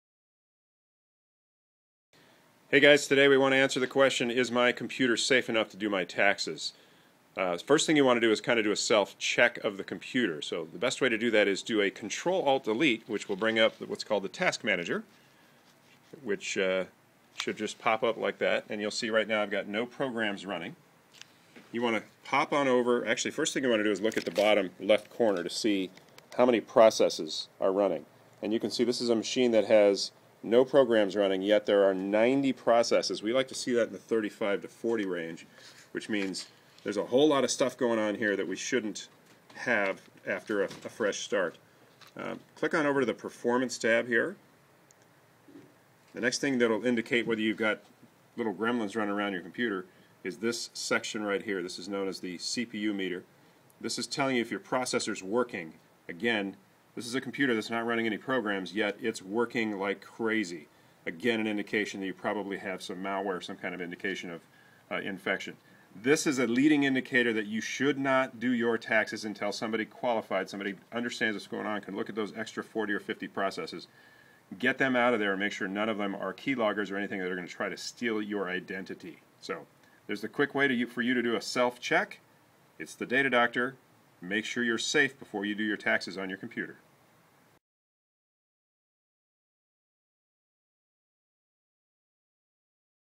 Is My Computer Safe Enough to Do My Taxes?
Tax software has become incredibly sophisticated and the choice for millions of US taxpayers to calculate and file their tax returns.

The popularity of doing your own taxes has created an opportunity for ID theft if you assume that your computer is risk free.

Here's a quick way to check your computer for any signs of infection or exploitation that would tell you whether you should begin preparing you tax return on your computer or not!